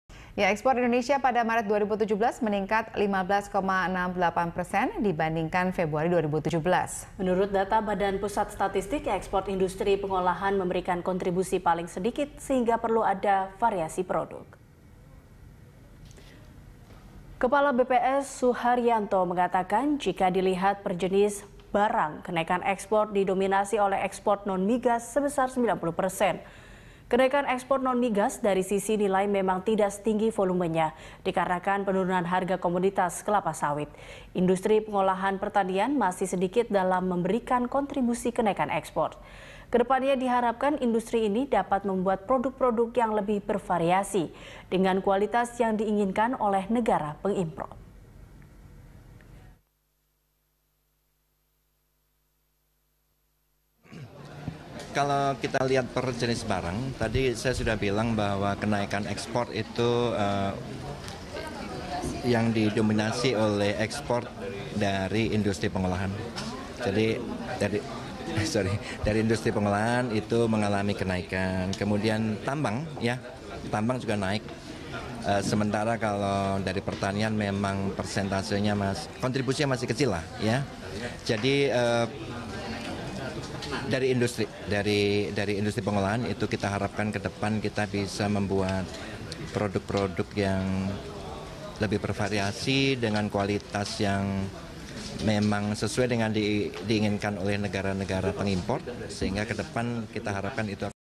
BPS: Maret 2017, Ekspor Indonesia Meningkat 15,68 Persen
Menurut data Badan Pusat Statistik, ekspor industri pengolahan memberikan kontribusi paling sedikit dalam ekspor Indonesia pada Maret 2017. Bulan ini ekspor mengalami peningkatan 15,68 persen dibandingkan Februari.

Facebook.com/BeritaSatuTV
Youtube.com/BeritaSatu